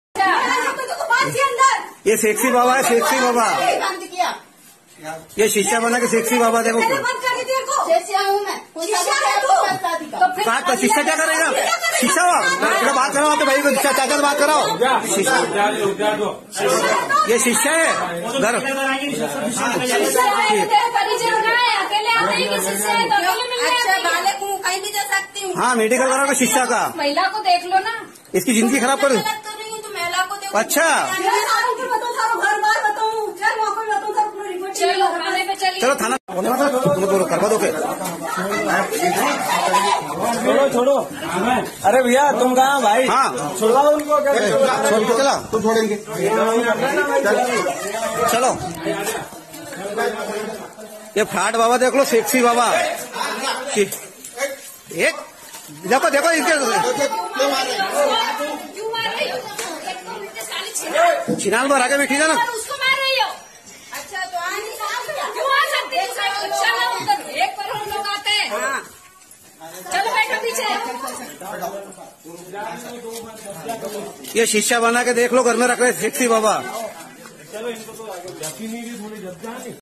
Viral Video: बंद कमरे में महिला शिष्या के साथ कथावाचक बाबा की अश्लील कथा, पुलिस ने किया गिरफ्तार
पत्नी ने कथावाचक जितेंद्र महाराज को दूसरी महिला के साथ पकड़ा। मामला मध्यप्रदेश के शाजापुर का है जहाँ कथावाचक महाराज की पत्नी रक्षाबंधन पर मायके गई थी, शक होने पर पुलिस के साथ घर पहुंची पत्नी ने दूसरी महिला के साथ पति को कमरे में पाया, दूसरी महिला ने खुद को बताया महाराज की शिष्या।